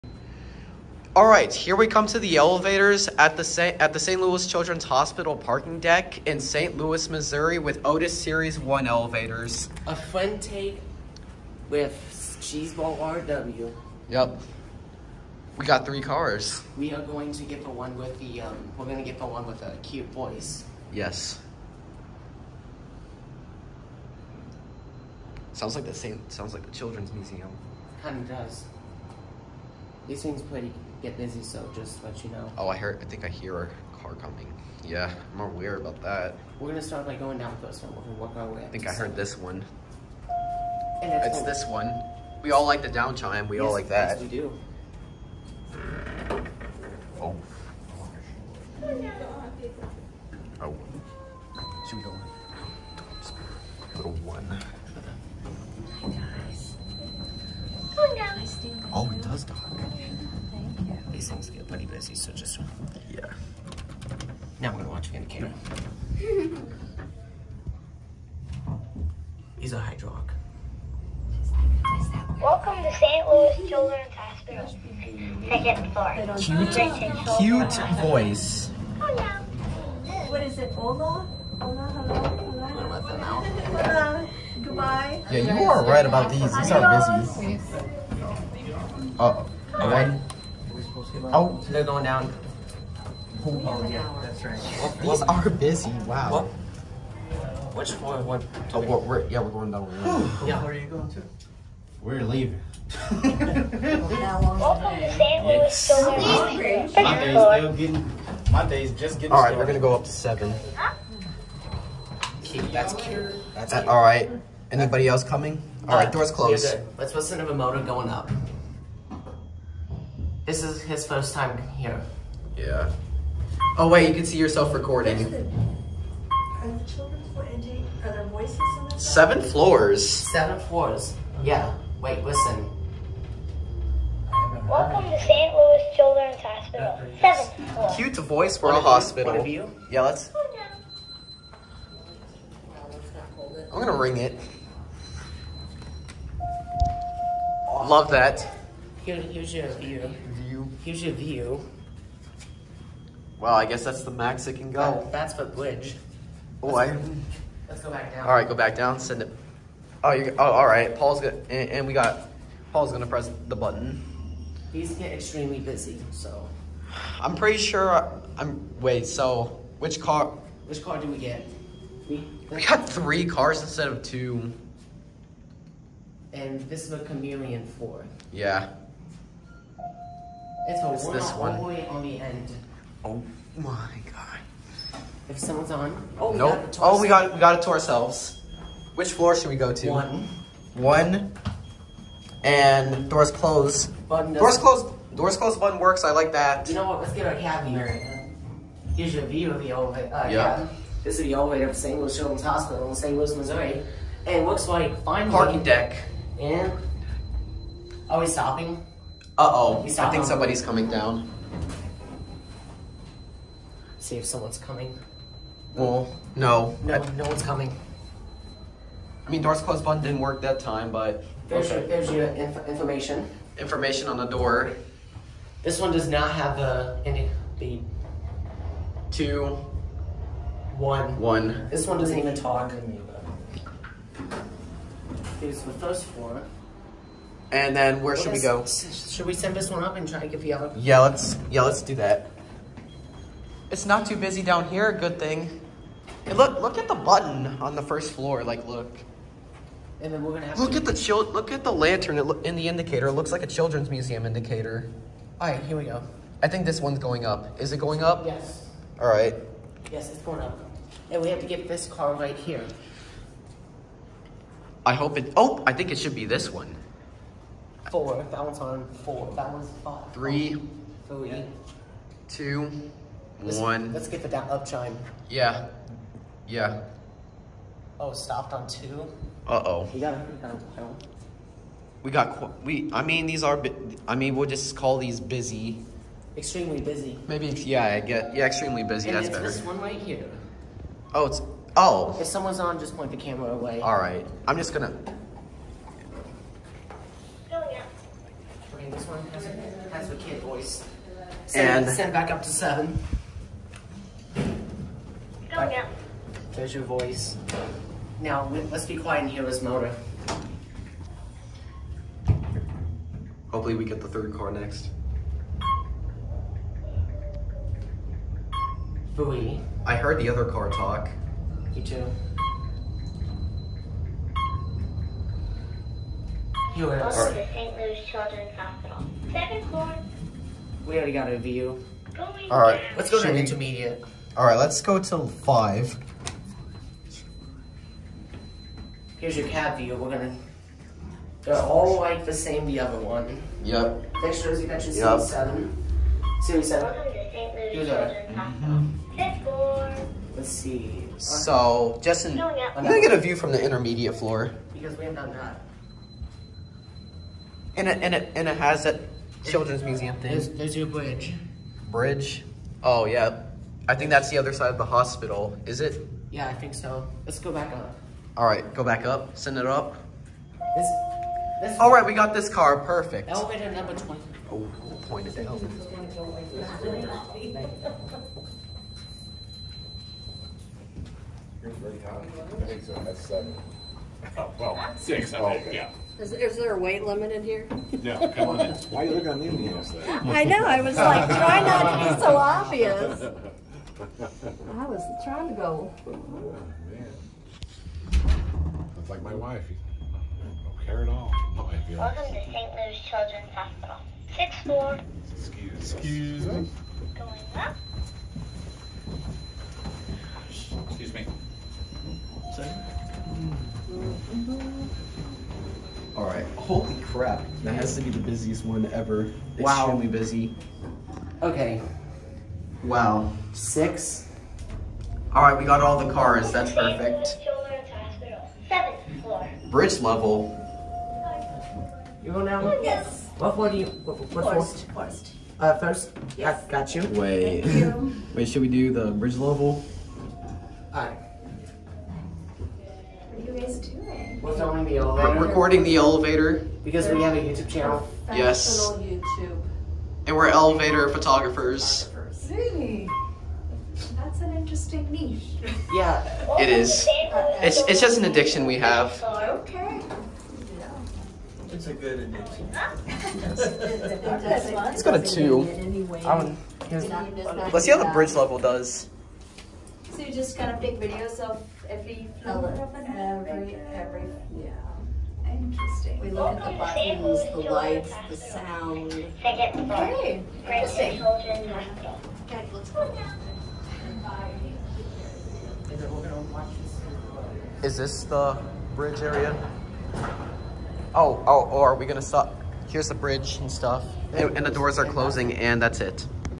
Otis Series 1 Hydraulic Elevators at Saint Louis Children’s Hospital/Parking Deck in Saint Louis MO
With my best friend ​⁠​⁠​⁠@toprocksss
These elevators get extremely busy, but at least got to ride them!

Elevator Specs
Button fixtures: Series 1
Motor: Hydraulic
Brand: Otis
Capacity: 4000 lbs
Floors served: 7